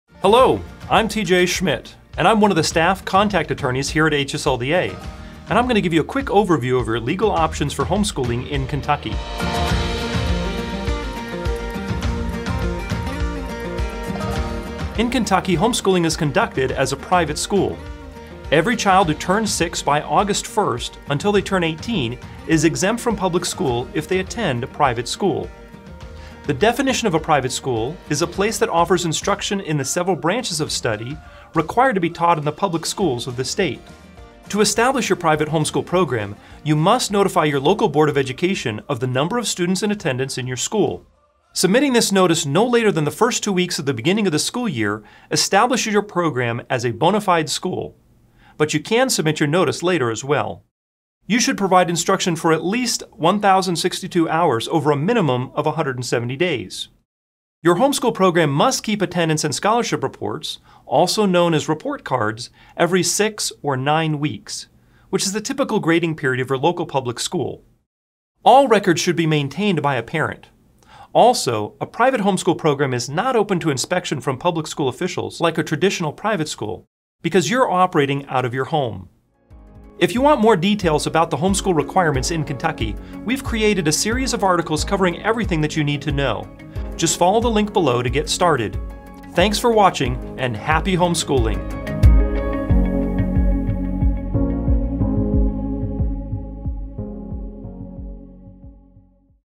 What’s the Homeschool Law in Kentucky? | A Quick Overview
If you'd like to learn more about the homeschool requirements in Kentucky, we've created a series of articles covering everything you need to know.

Please note: Although HSLDA is a legal advocacy organization, nothing in this video, even legal information, can be considered actual legal advice.